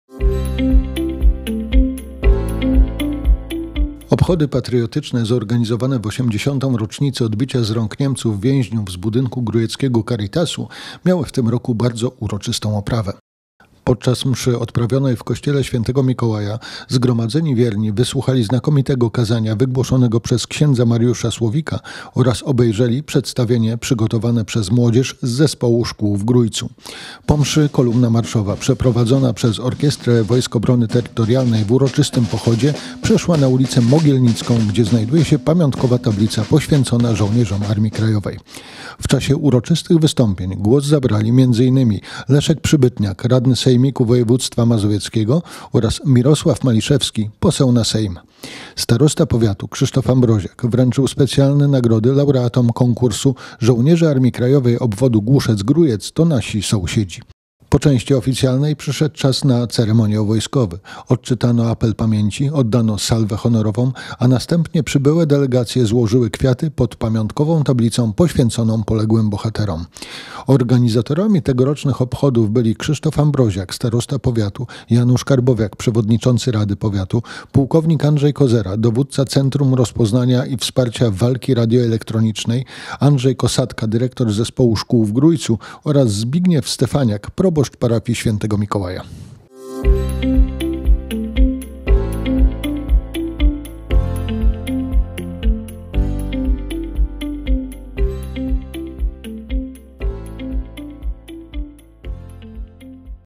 Obchody patriotyczne w Grójcu
Obchody patriotyczne zorganizowane w 80. rocznicę odbicia z rąk Niemców więźniów z budynku Grójeckiego Caritasu, miały w tym roku bardzo uroczysta oprawę.
Podczas mszy, odprawionej w kościele świętego Mikołaja, zgromadzeni wierni wysłuchali znakomitego kazania wygłoszonego przez księdza Mariusza Słowika oraz obejrzeli przedstawienie przygotowane przez młodzieży z Zespołu Szkół w Grójcu.
Po mszy kolumna marszowa prowadzona przez orkiestrę wojsk Obrony Terytorialnej w uroczystym pochodzie przeszła na ulicę Mogielnicką, gdzie znajduje się pamiątkowa tablica poświęcona żołnierzom Armii Krajowej.
W czasie uroczystych wystąpień głos zabrali m.in. Leszek Przybytniak - radny sejmiku województwa Mazowieckiego oraz Mirosław Maliszewski - poseł na Sejm.
Starosta powiatu - Krzysztof Ambroziak - wręczył specjalne nagrody laureatom konkursu "Żołnierze Armii Krajowej Obwodu "Głuszec" Grójec, to nasi sąsiedzi...".
Po części oficjalnej przyszedł czas na ceremoniał wojskowy. Odczytano apel pamięci, oddano salwę honorową a następnie przybyłe delegacje złożyły kwiaty pod pamiątkową tablicą, poświęconą poległym bohaterom.
Organizatorami tegorocznych obchodów byli: Krzysztof Ambroziak - starosta powiatu, Janusz Karbowiak - przewodniczący rady powiatu, płk. Andrzej Kozera - dowódca Centrum Rozpoznania i Wsparcia Walki Radioelektronicznej, Andrzej Kosatka - dyrektor Zespołu Szkół w Grójcu oraz Zbigniew Stefaniak - proboszcz parafii św. Mikołaja.